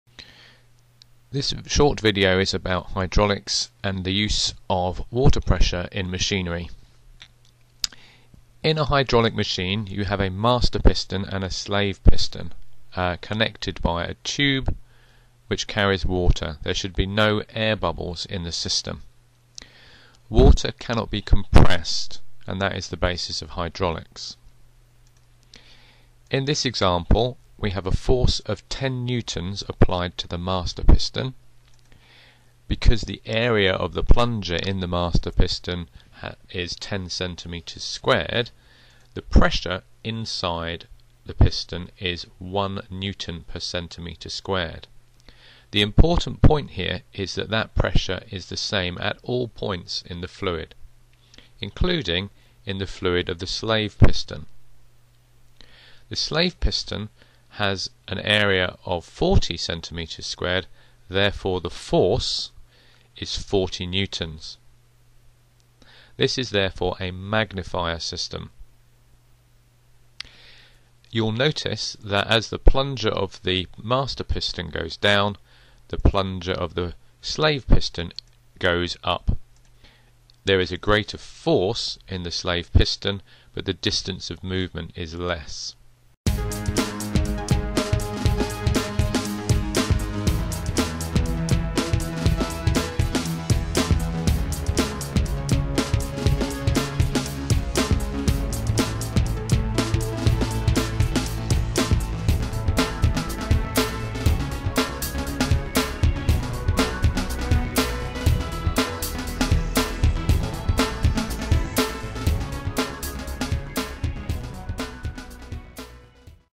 Hydraulics
A simple hydraulic system explained - aimed at KS3 science pupils